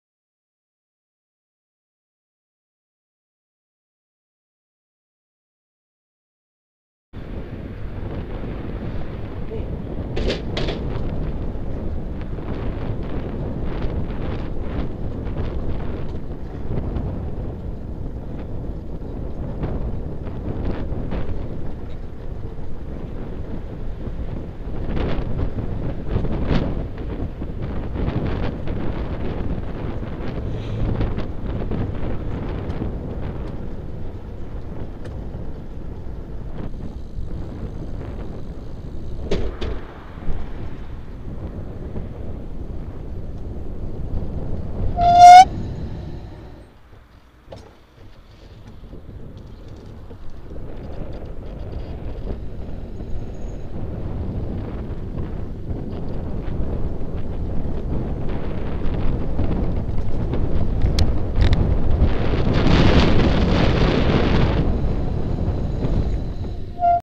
Near head on collision in my Rotovelo velomobile
On a ride when on a blind corner some idiot overtook a man walking a dog and nearly ran into the front of me.  Not a word came out of his mouth.